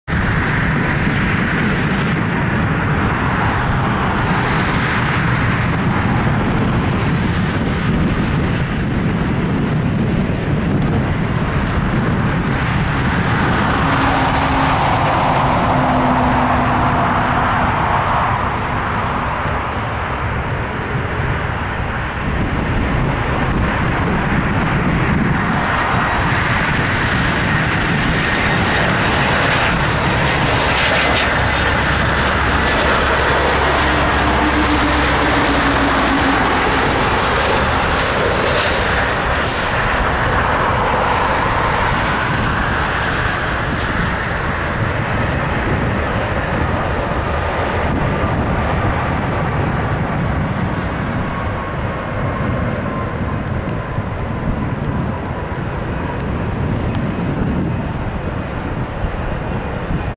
Virgin America (Dark Horse) N524VA A319 Landing Portland Airport (PDX)
Flight: VRD925 SFO-PDX